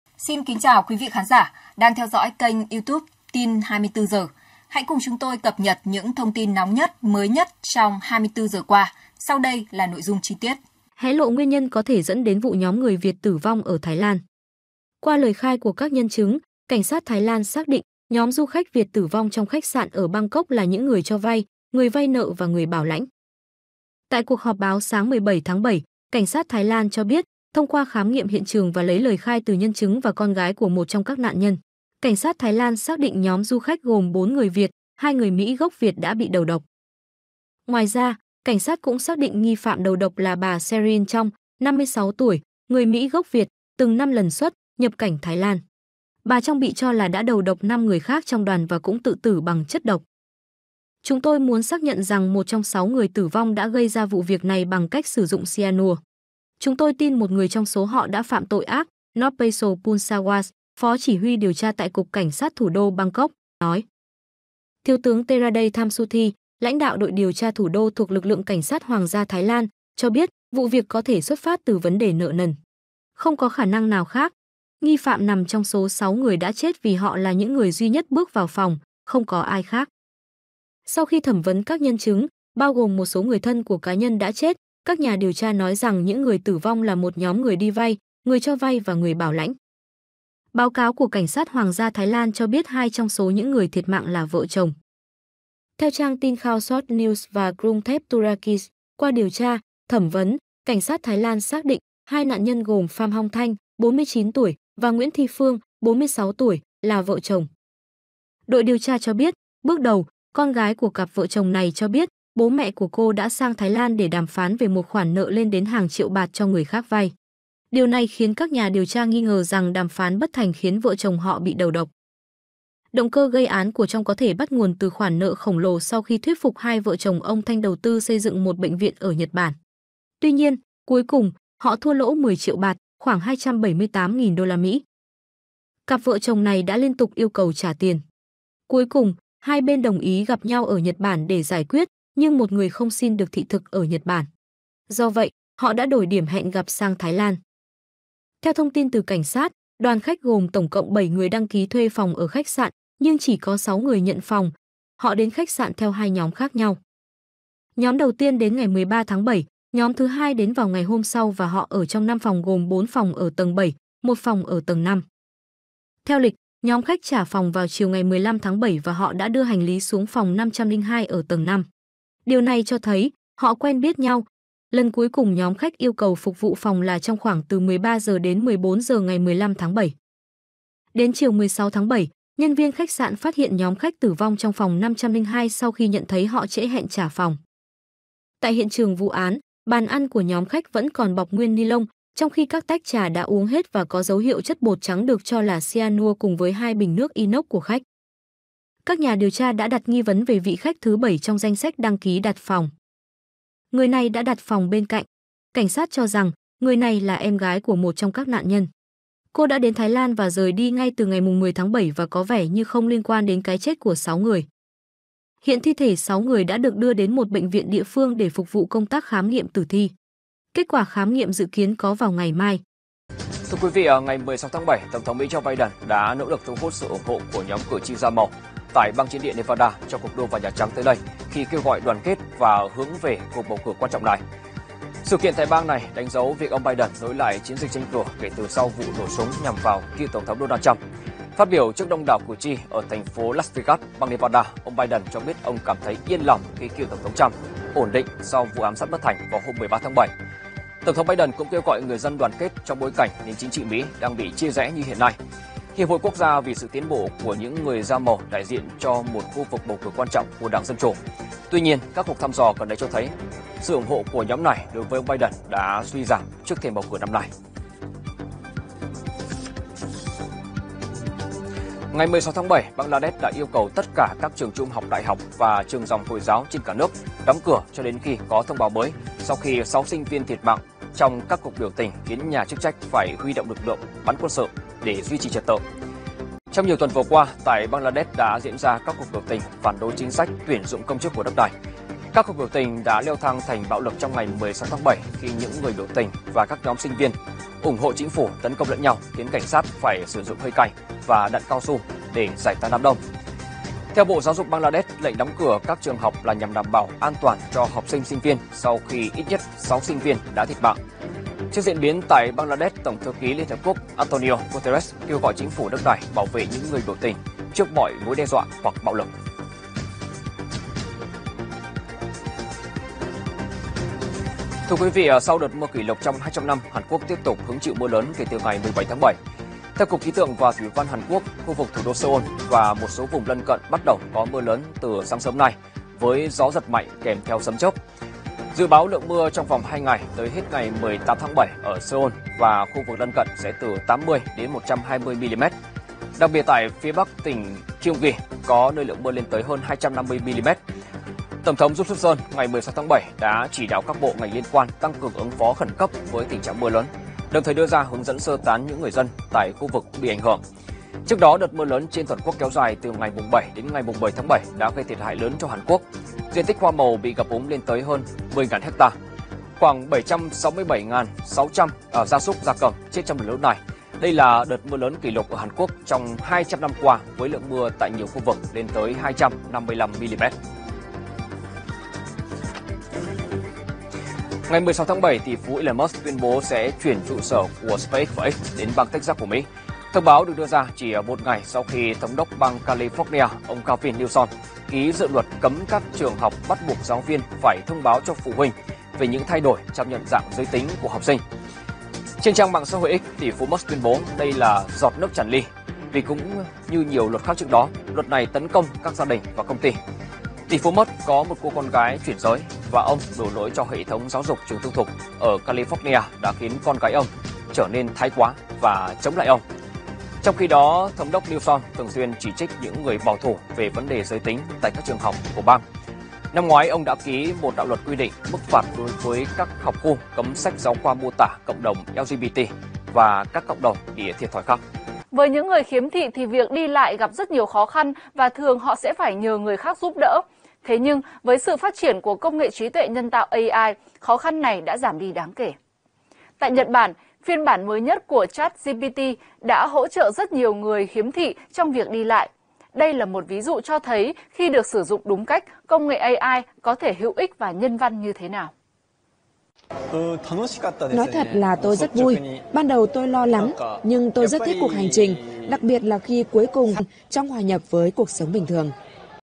Vụ 6 người Việt tử vong ở Thái Lan: Con gái nạn nhân hé lộ về khoản nợ "khủng" | Tin24h
Liên quan đến vụ 6 người Việt tử vong ở Thái Lan, báo Tuổi trẻ Online dẫn nguồn tin từ Bangkok Post, tại đồn cảnh sát Lumpini sáng 17/7, Thiếu tướng Theeradet Thumsuthee cho biết cảnh sát tin rằng một người trong số các nạn nhân đã đầu độc những người còn lại rồi tự sát. Cảnh sát cũng thẩm vấn con gái của một trong những nạn nhân cùng các nhân chứng khác, và tài khoản ngân hàng của họ rất có ích cho việc điều tra.

Theo nội dung trao đổi với thân nhân của những người thiệt mạng, đã có tranh chấp về nợ nần giữa họ. Tướng Noppasilp Poonsawat, phó cảnh sát trưởng Bangkok, cho biết đang điều tra kỹ nguyên nhân một số người trong các nạn nhân đã nhiều lần xuất - nhập cảnh Thái Lan.

Cũng theo cảnh sát, hai nạn nhân Pham Hong Thanh và bà Nguyen Thi Phuong là vợ chồng, có một công ty cầu đường ở Việt Nam. Con gái của cặp vợ chồng cho biết cha mẹ mình đến Thái Lan để đàm phán về số tiền hàng triệu baht cho người khác mượn. Điều này khiến các quan chức tin rằng do không thể đàm phán để giải quyết khoản nợ nên hai vợ chồng đã bị đầu độc.